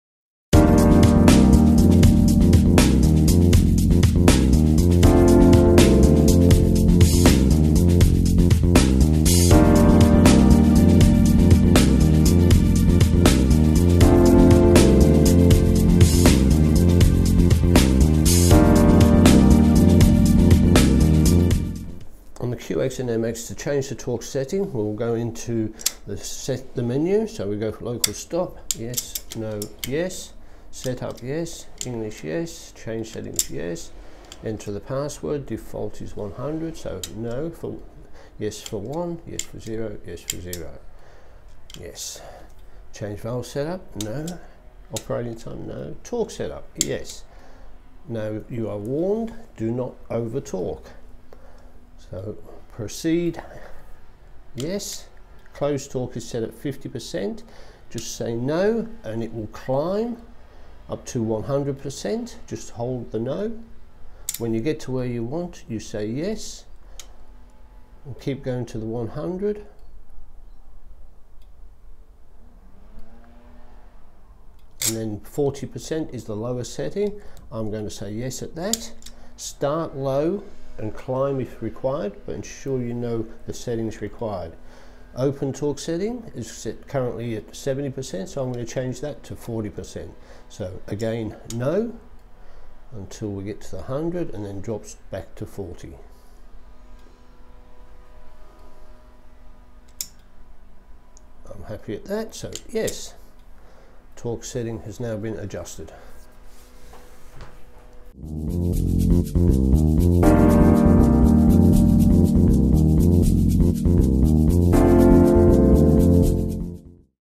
Changing Torque Settings on a Limitorque MX/QX electric actuator
In this episode of Acrodyne TV we show you how to change the torque settings on a Limitorque MX/QX electric actuator.

The Limitorque MX electric actuator can be used for on/off, position and modulating control with a wide range of control options mostly select able through the simple yes/no menu from the Hall Effect switches on the actuator cover.

State of the art intelligent valve control offering Limitorque’s electric actuator reliability along with the simplicity of non intrusive set up. No remote controls to loose. Limitorque MX electric actuator use absolute encoders for position sensing and require no battery back up to maintain position settings through loss of power and operating manually.

The Limitorque QX quarter-turn, smart electric valve actuator mimics the state of the art, non-intrusive, multi-turn MX electric actuator by including a patent pending absolute encoder for tracking position without the use of troublesome batteries. The Limitorque QX electric actuator design provides enhanced safety and reduced downtime through improved diagnostics, built in self test (BIST) features and LimiGard™ fault protection.